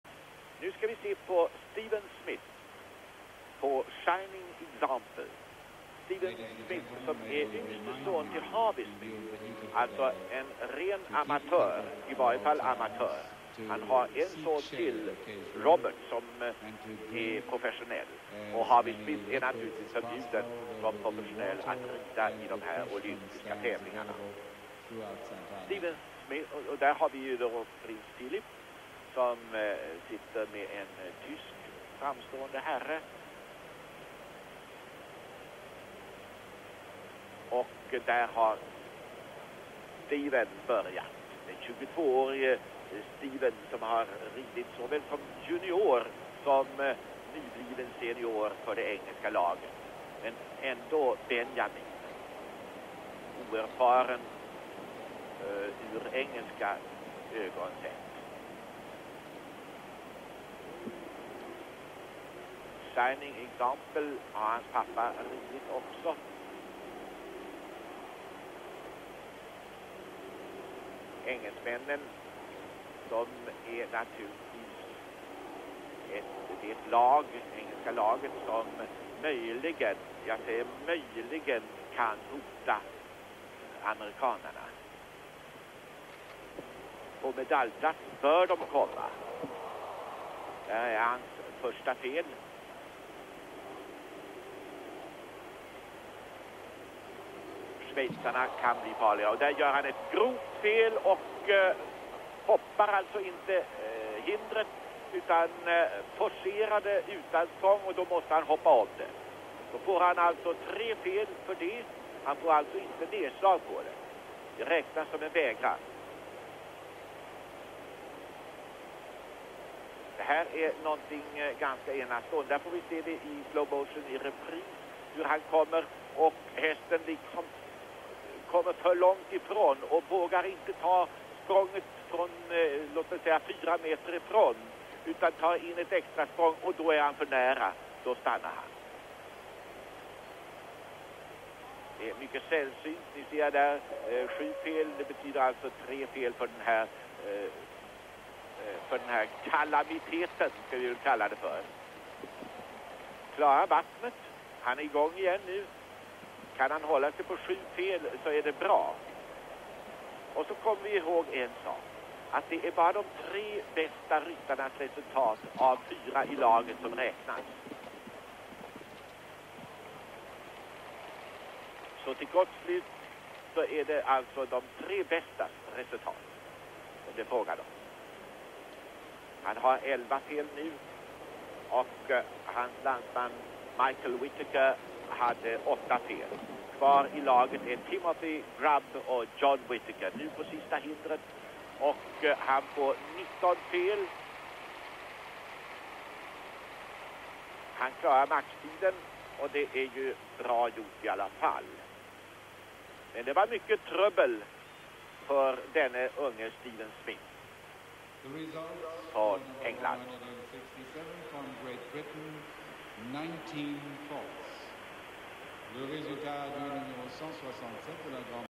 Steven Smith - Shining Example - LA 1984
Show jumping Los Angeles 1984 - Team 1st round.